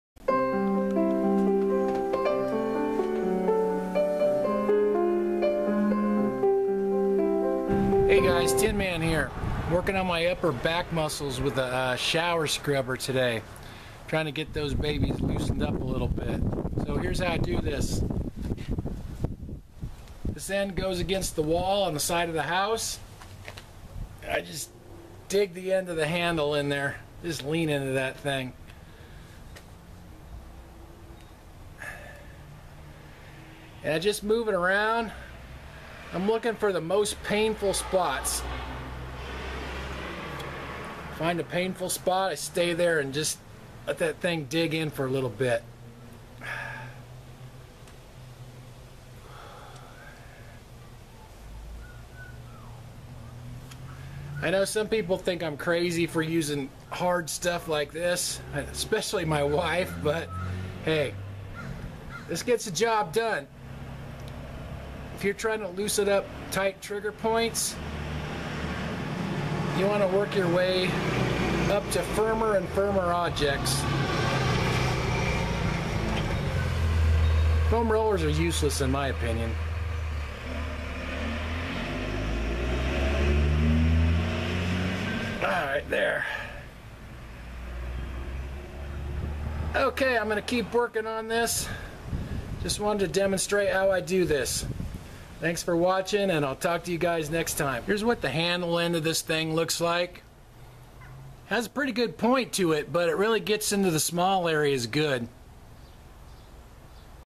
Thoracic Outlet Syndrome-Releasing Tight Upper Back Muscles
Thoracic Outlet Syndrome and tight upper back muscles seem to go hand in hand, for me anyway. Here I'm demonstrating some self-myofascial release on some tight upper back muscles. Thoracic outlet syndrome causes a lot of muscle pain so I spend a lot of time doing trigger point work. Self-myofascial release is a big part of my pain management routine. Tight muscles and trigger points can be very painful.